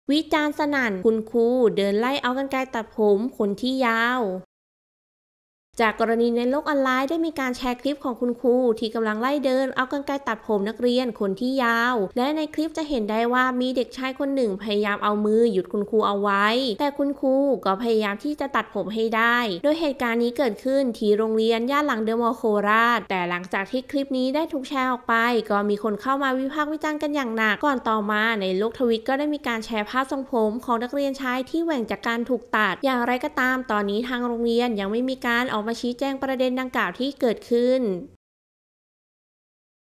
วิจาร์ณสนั่น ครูเดินไล่เอากรรไกรตัดนักเรียน
จากกรณีโลกออนไลน์ ได้มีการแชร์ภาพขอบคุณครู ที่กำลังไล่เดิน เอากรรไกรตัดผมนักเรียนคนที่ผมยาวและในคลิปจะเห็นได้ว่า มีเด็กคนหนึ่งพยายามเอามือหยุดคุณครูไว้